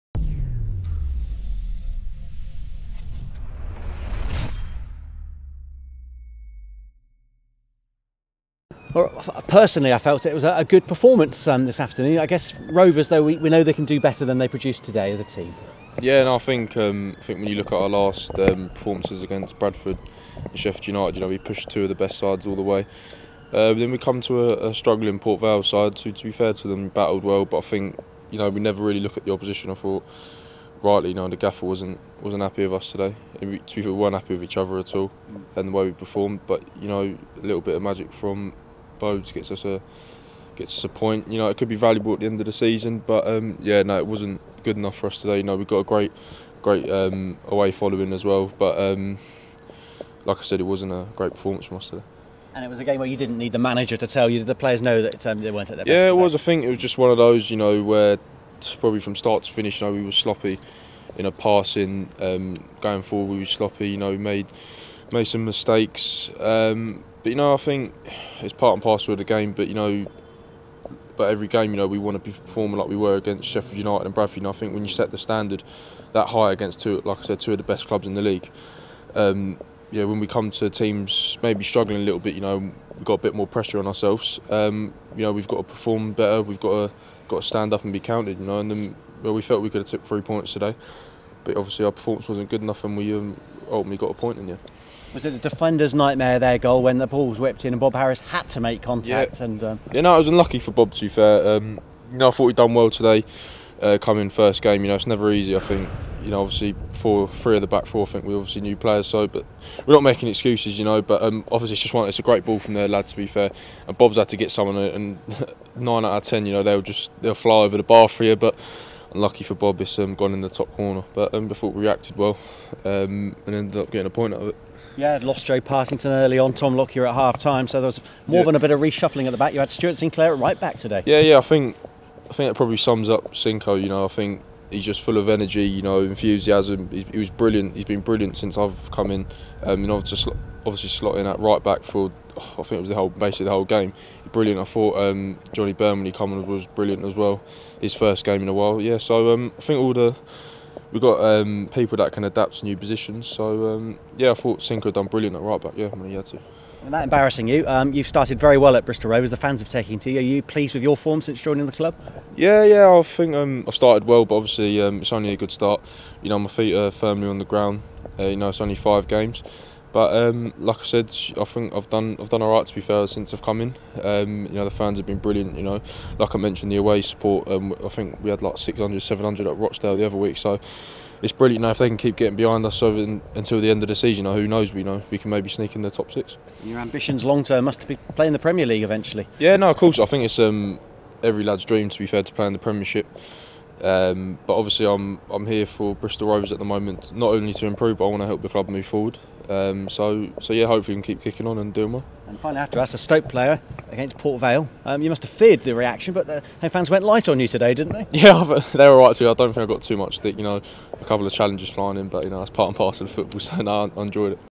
REACTION: Ryan Sweeney post Port Vale
Bristol Rovers defender Ryan Sweeney talks about yesterday's 1-1 draw with Port Vale and settling in at the club.